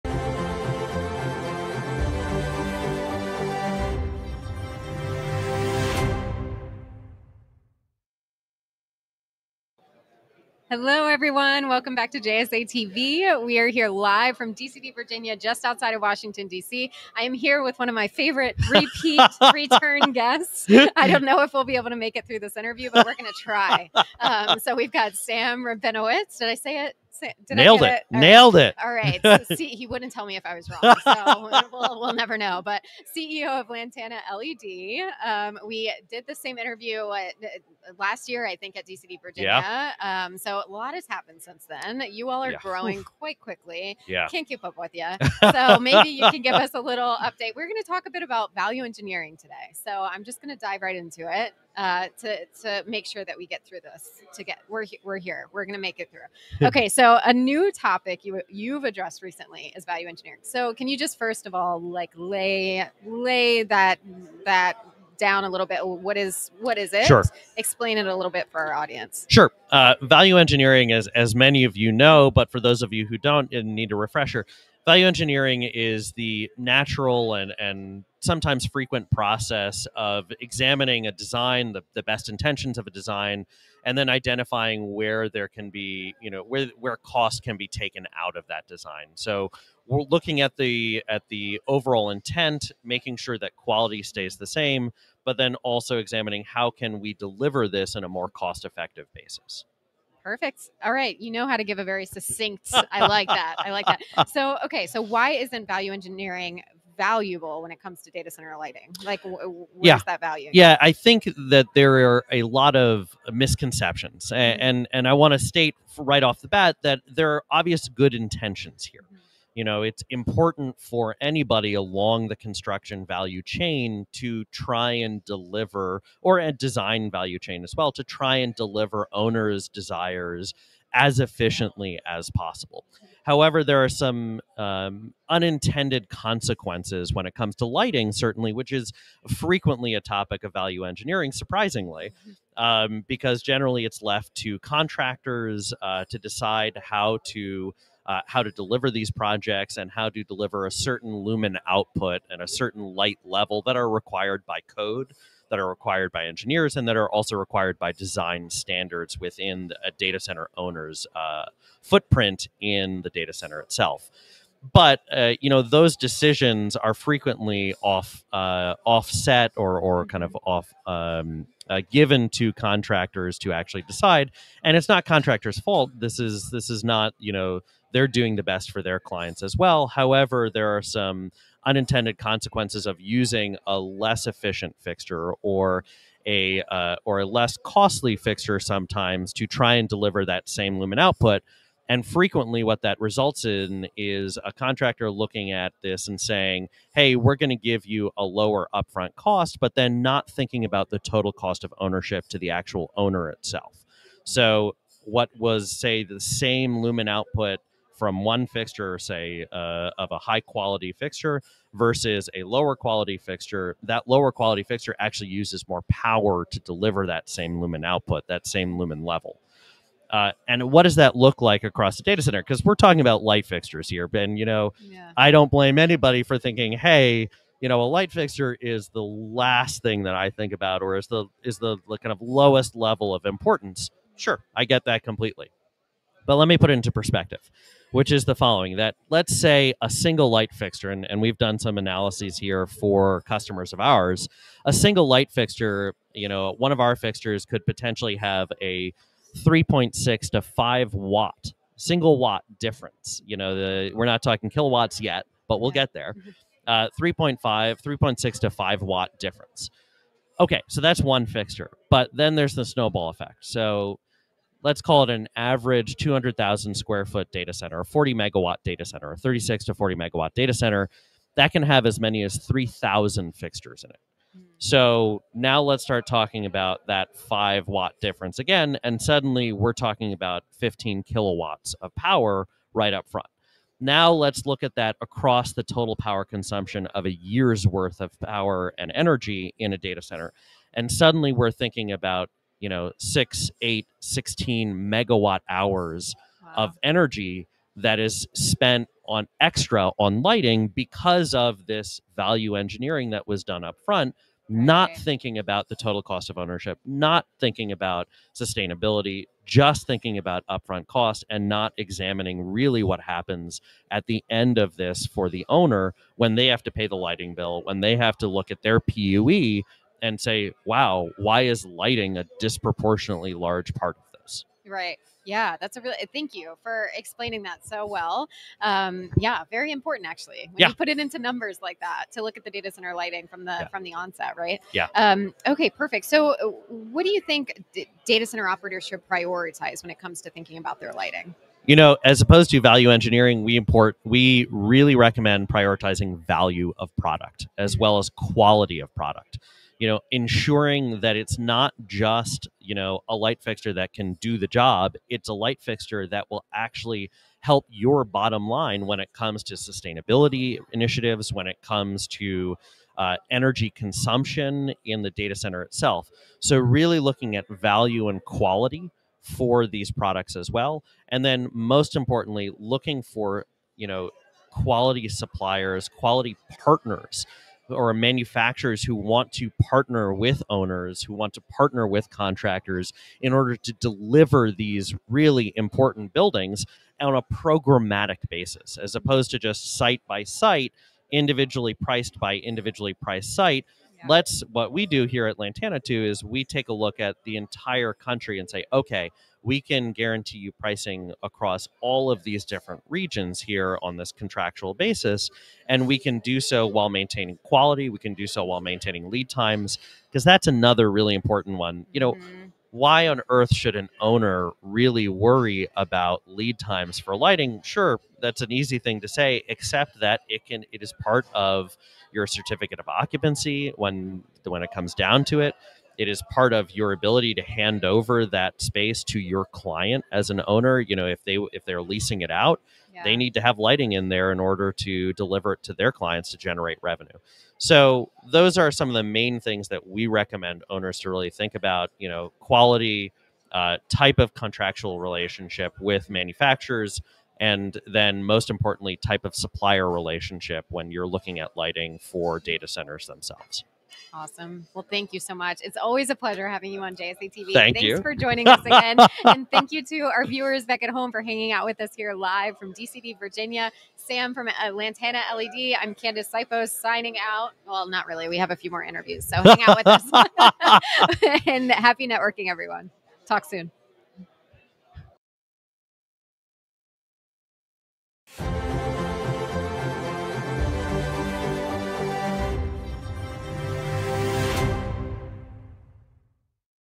DCD Virginia 2024 | LANTANA LED's Sam Rabinowitz Talks Value Engineering in Data Center Lighting
What is value engineering and should you consider it for your data center lighting solutions? In this interview at DCD Virginia, LANTANA LED CEO Sam Rabinowitz explains the idea of value engineering, why it may not lead to cost or energy savings when it comes to lighting and what data center operators should prioritize when it comes time to pick their lighting.